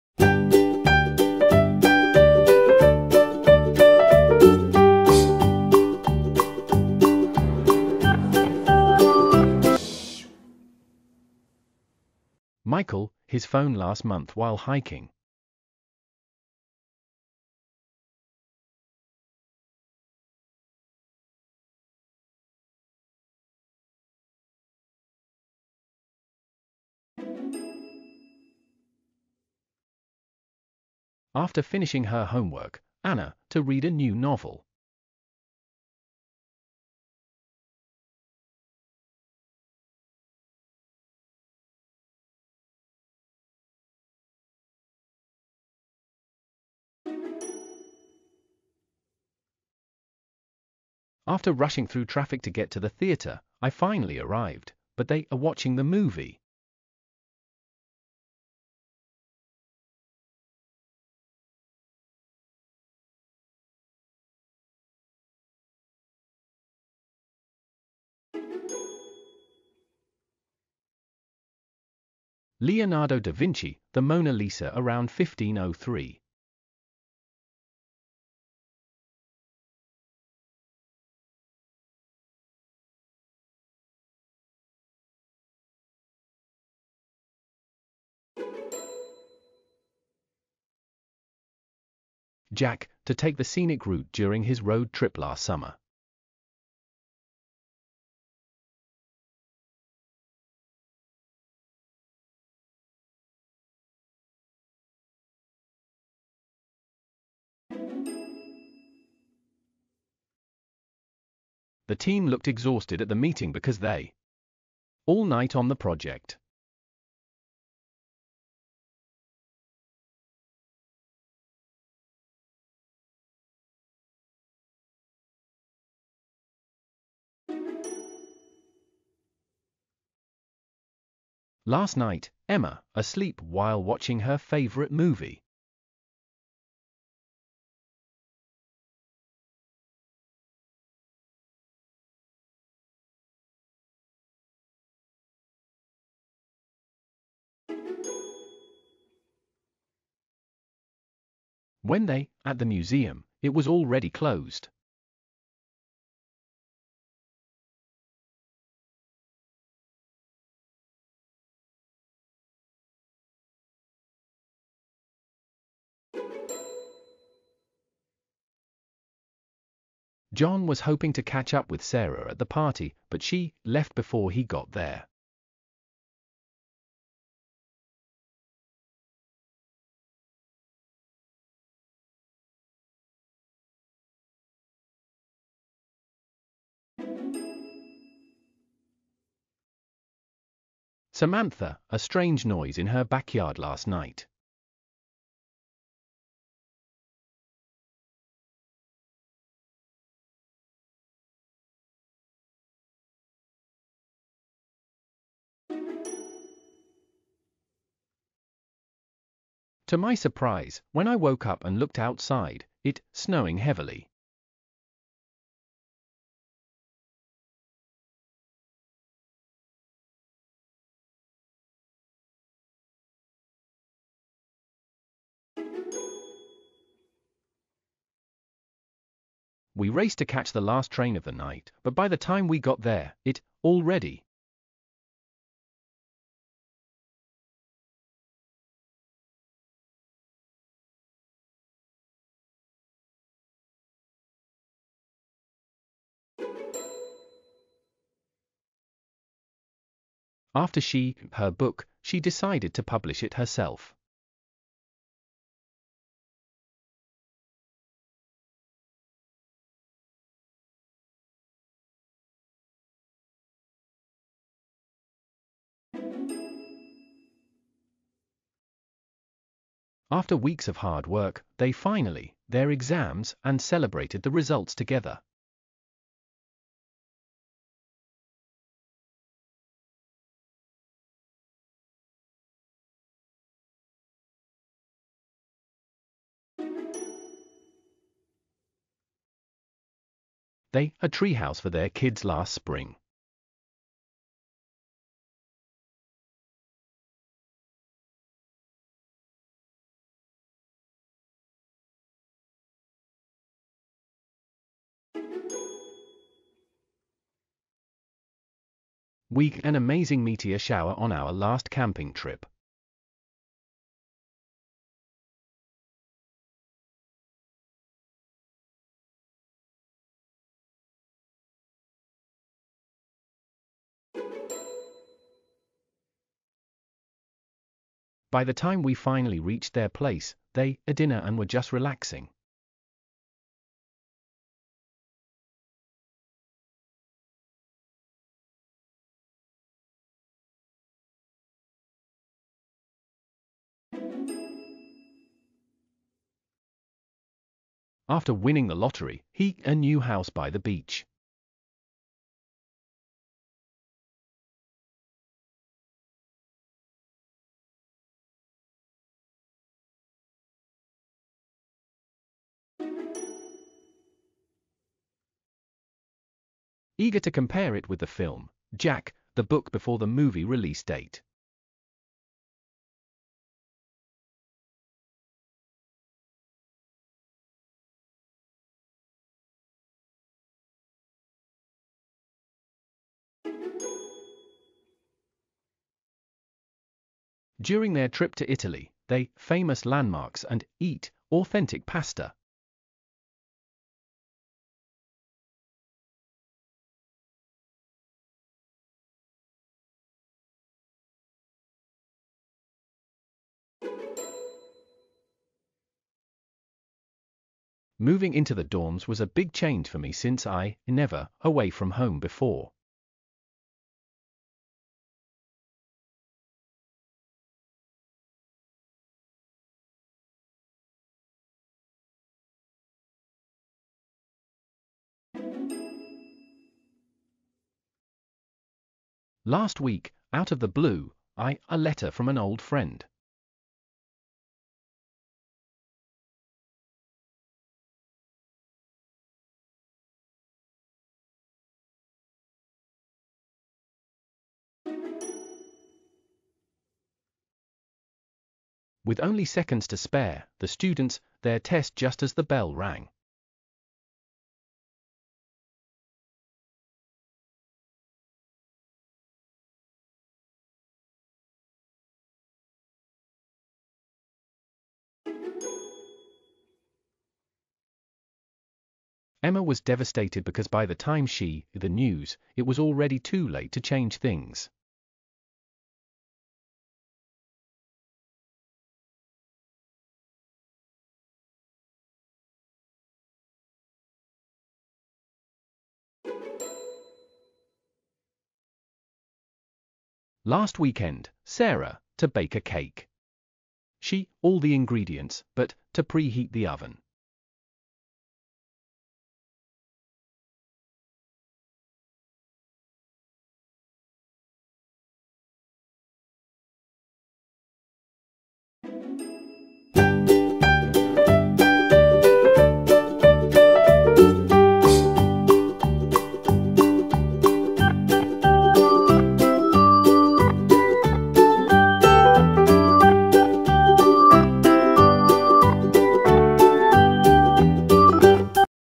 PAST SIMPLE or PAST PERFECT? Are You an English Tenses Expert? Take the Quiz! | Nanda
The past simple and past perfect are both used to describe actions in the past, but they serve different purposes.
The past simple is used for completed actions that happened at a specific time, often with time markers like "yesterday," "last year," or "in 2010." For example, "She visited Paris last summer." This tells us when the action happened, and it’s straightforward.
The past perfect, however, is used to show that one past action happened before another past action. It emphasizes the order of events. For example, "She "had visited" Paris before moving to London." Here, the past perfect ("had visited") highlights that visiting Paris happened before the past simple action of moving to London. In short, while the past simple reports a past event, the past perfect adds context by showing what happened earlier in the past.